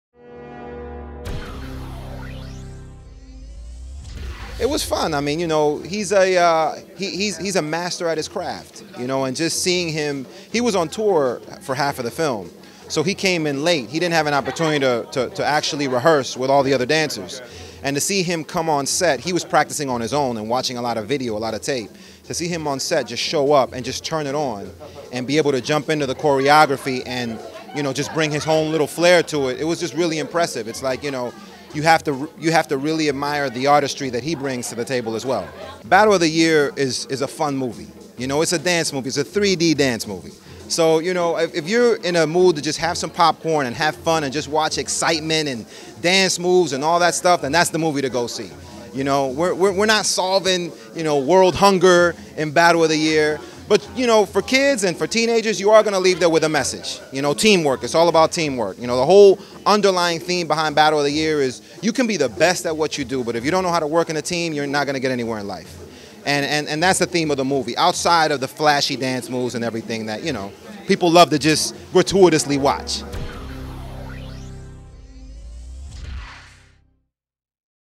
Laz Alonso Talks Working With Chris Brown on the Set of 'Battle of the Year' - HipHollywood.com
HipHollywood caught up with Laz Alonso to talk about his new film, 'Battle of the Year,' which also features Chris Brown. Alonso raves about Brown's dance skills and work ethic, calling him "a master of his own craft."